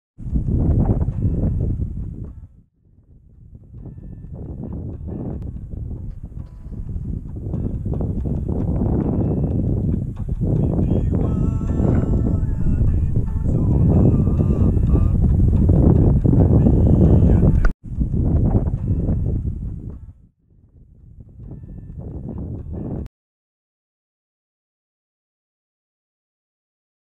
CDT Sign Riddled with Bullet Holes (2019 CDT SOBO Thru-Hike)
This really added to the stereotypical desert ambiance. A dry and barren dust bowl, a few shrubs, rusted out tin cans, and anything that can be used as target practice...is used as target practice